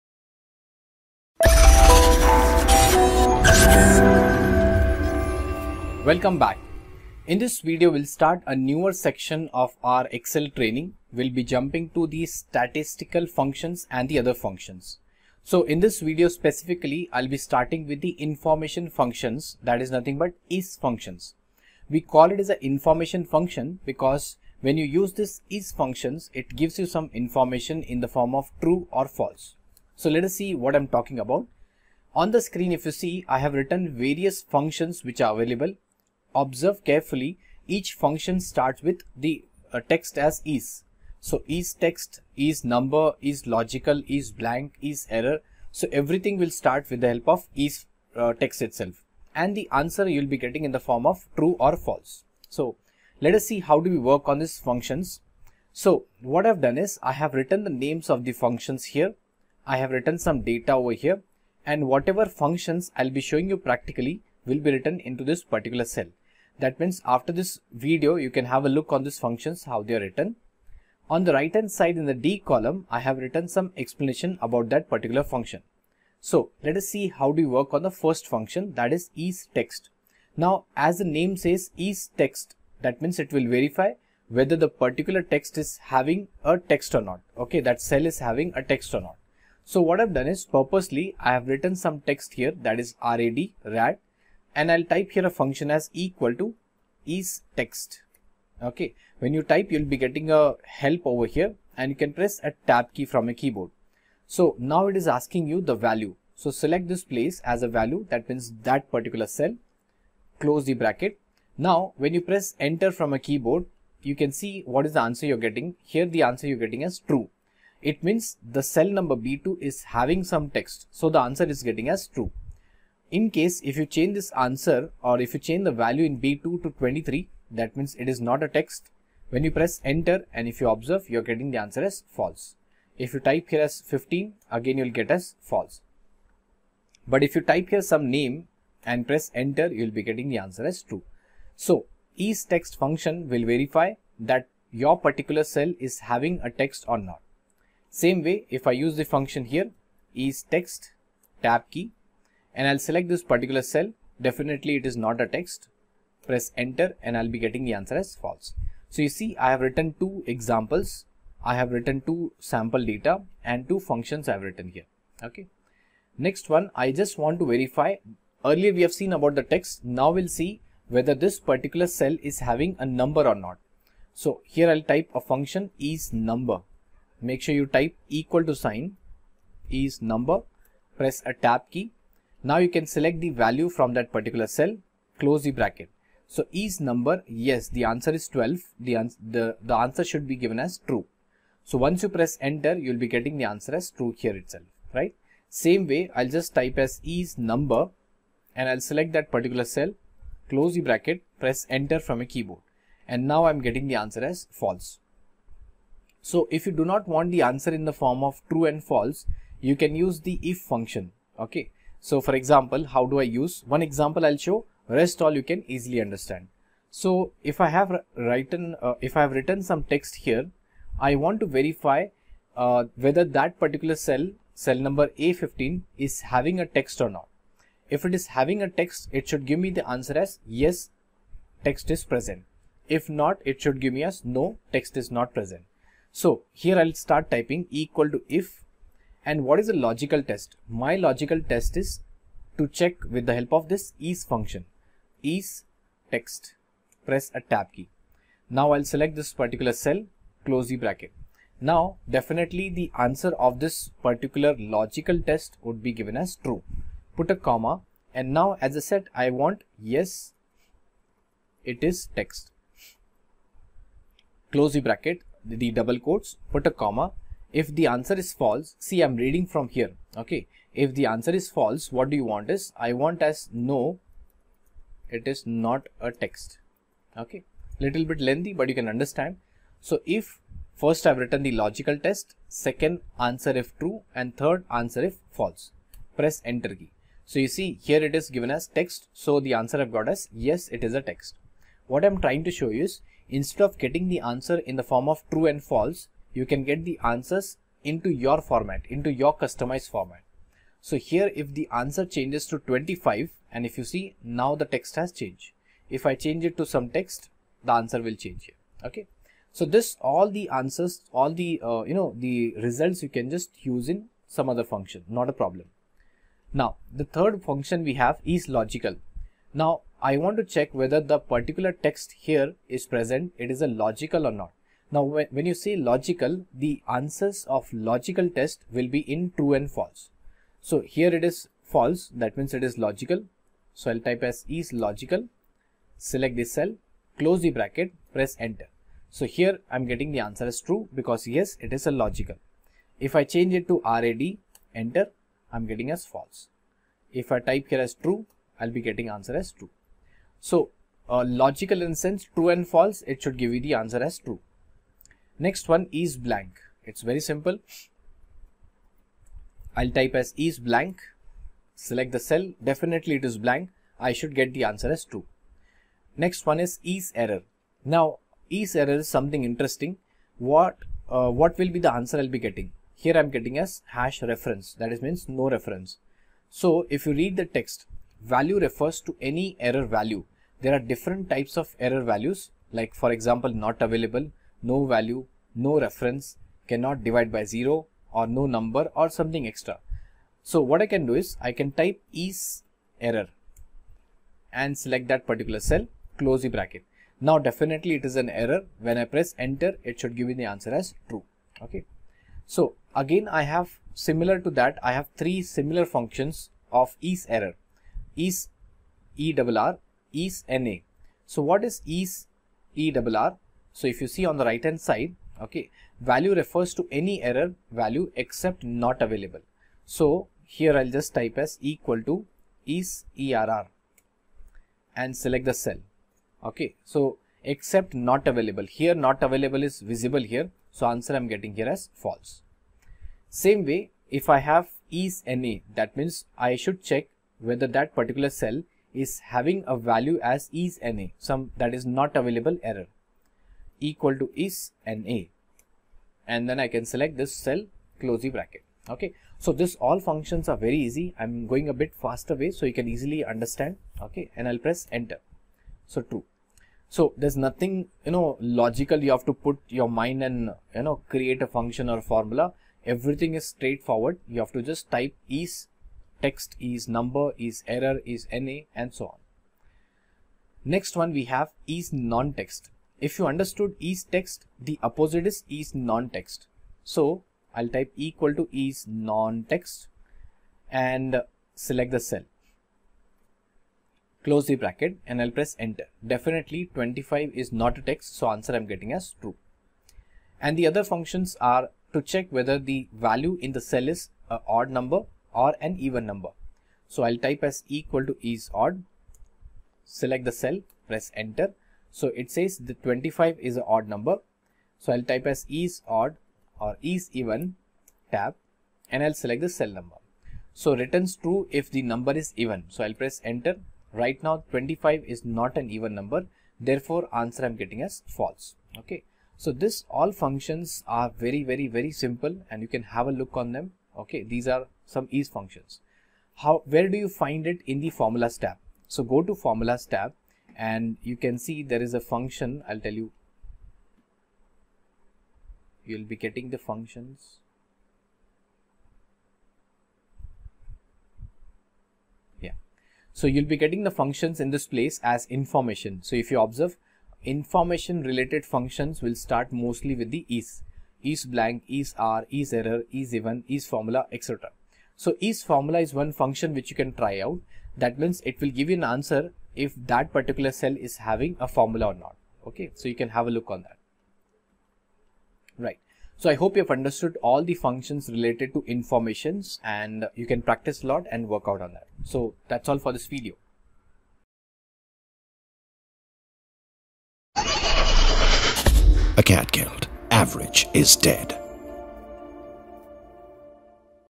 Excel Tutorial for Beginners #11 | Excel Formulas and Functions | Information Function in Excel
Welcome back to MS Excel Tutorial for Data Analytics powered by Acadgild. In the previous session, we talked about Data Validation in Excel. In this excel tutorial, you will be able to see a newer section of excel training, will be jumping through the statistical functions and other functions. Particularly in this video, you will be able to start with information functions in Excel that is nothing but ‘IS’ function. We call this as information function because when we use this function it gives information in the form of true or false.
Information functions in Excel:
1. ISTEXT function
2. ISNUMBER function
3. ISLOGICAL function
4. ISBLANK function
5. ISERROR function
6. ISERR function
7. ISNA function
8. ISODD function
9. ISEVEN function